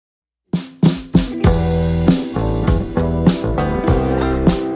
Wall Target Drill
A thrust hitting the center dot on the target.